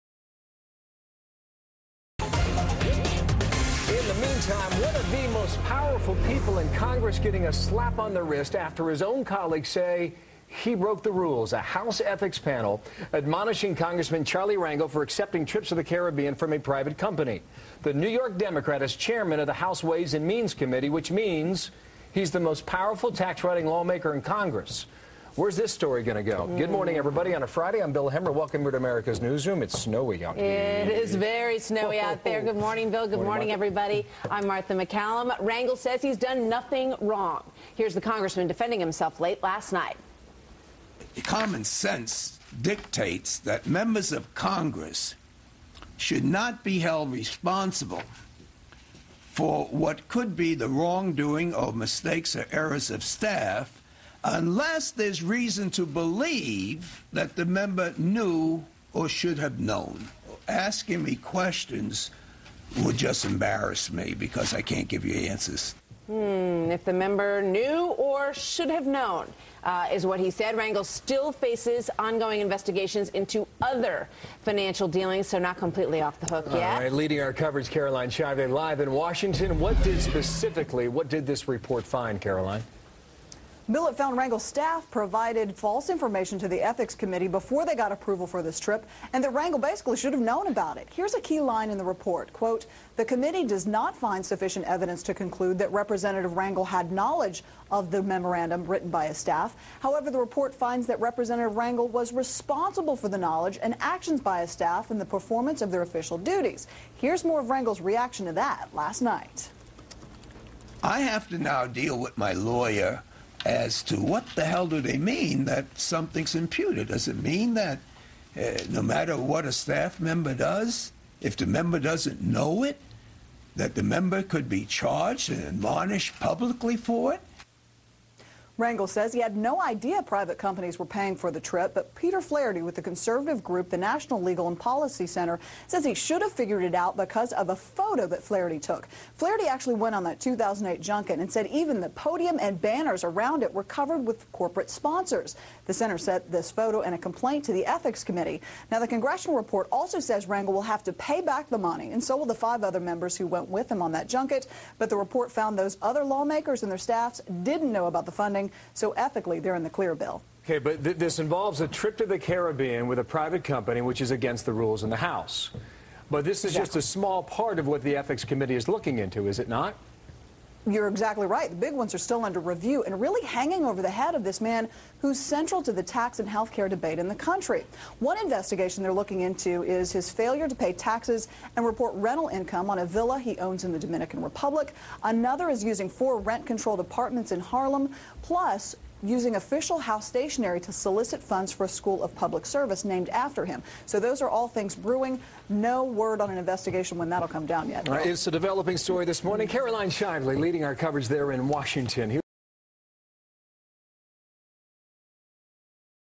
NLPC Exposed Charles Rangel Caribbean Junket
Fox News Channel reports on the House Ethics Committee's admonition of Ways and Means Chairman Charles Rangel (D-NY) for accepting trips to Caribbean paid for by Citigroup and other corporations. NLPC President Peter Flaherty attended 2008 St. Maarten event and made photographs and recordings showing corporate involvement.